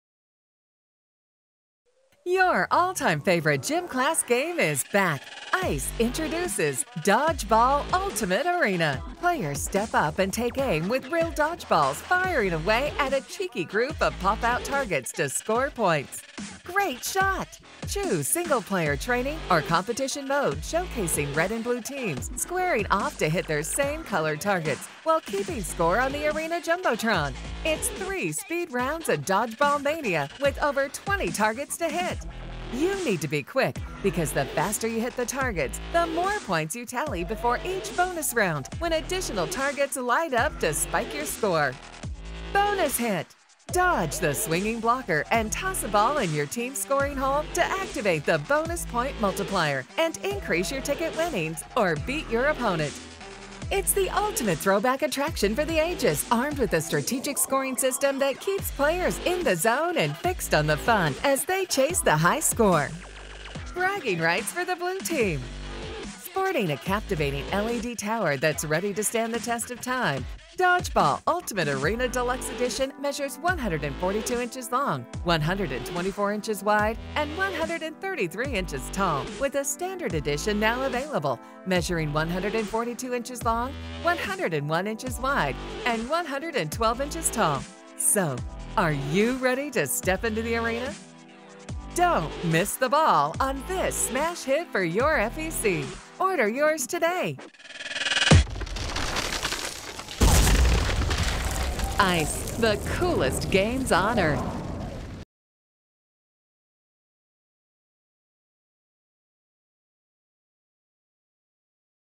Dodgeball Ultimate Arena | Sega Amusements
Dodgeball is a highly interactive ball toss skillbased redemption game. Players must aim at random pop out targets throughout gameplay along with stationary holes to score points which translate to tickets. The quicker the target is hit the more points are awarded.